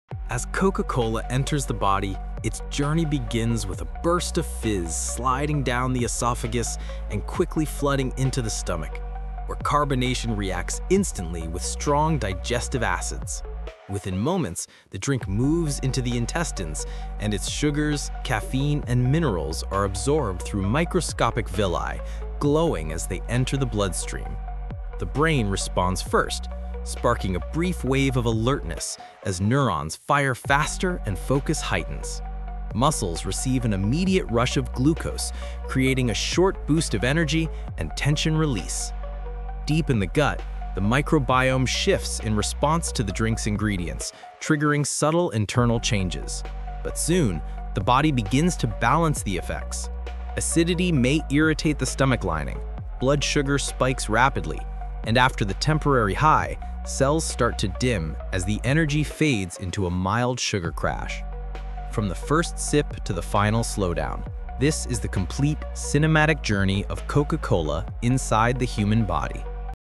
😲Coca-Cola Effects on the Body: How Sugary Drinks Impact Your Health
Discover what happens inside your body when you drink Coca-Cola. From the first sip to sugar and caffeine absorption, learn how this popular soft drink affects your stomach, intestines, heart, brain, and overall energy. Perfect for health-conscious viewers and anyone curious about the science behind sugary beverages.